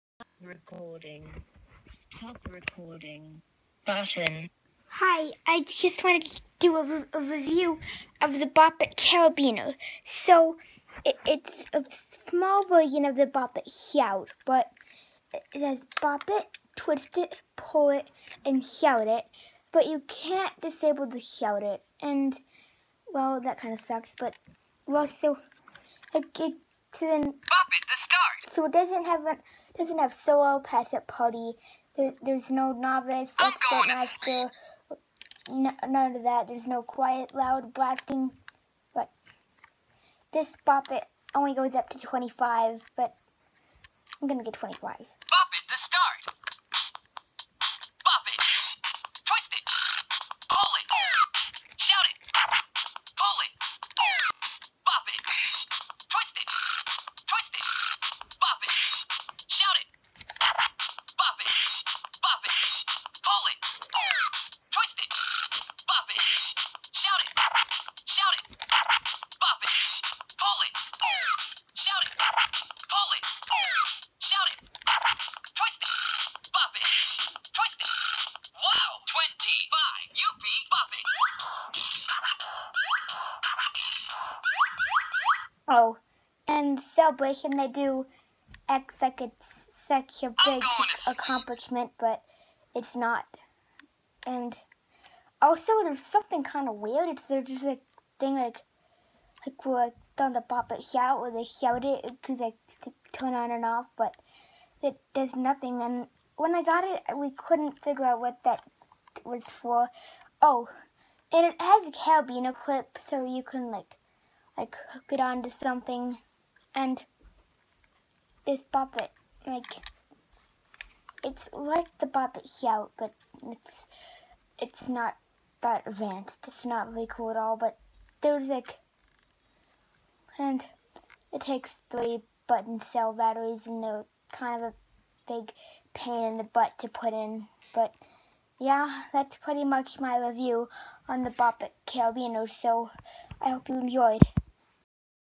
Bop it carabiner review
This is my bucket carabiner